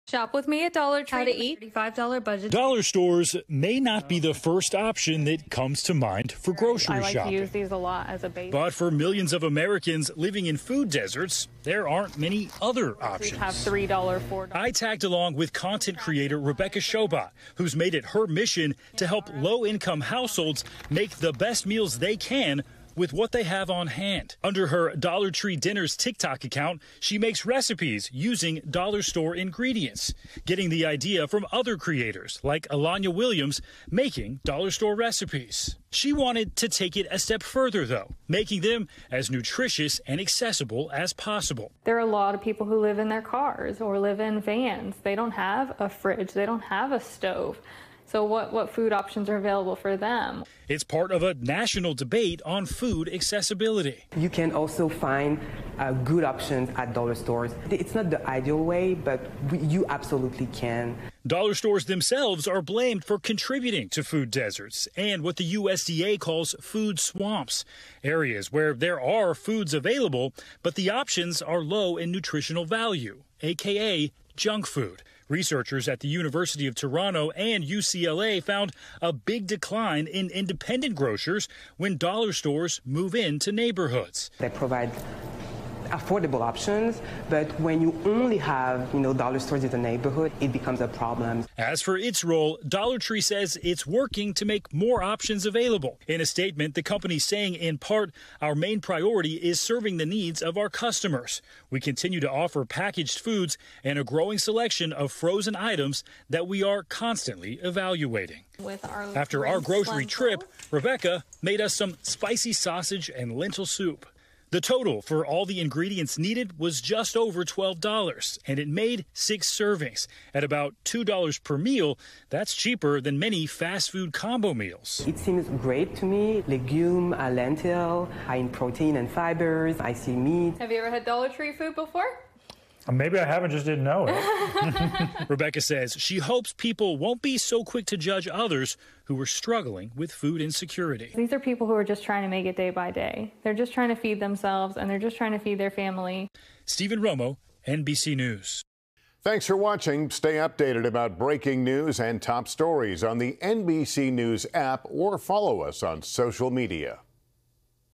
How to create a well-balanced meal from dollar store groceries
NBC's Steven Romo speaks with content creator Rebecca Chobat who details how to effectively shop for groceries at a dollar store for Americans living in food deserts. She uses her platform to help low-income households make the best meals they can with fewer resources.